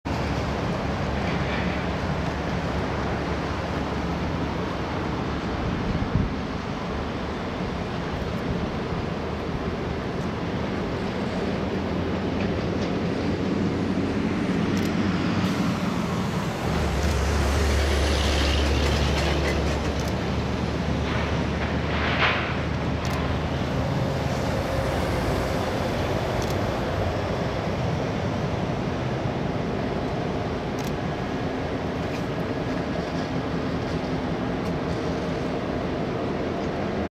BNSF GP38 / GP38-2R # 2236 crew rides past @ Vancouver, WA 6/24/15
BNSF GP38 / GP38-2R # 2236 crew rides past @ Vancouver, WA working next assignement 00039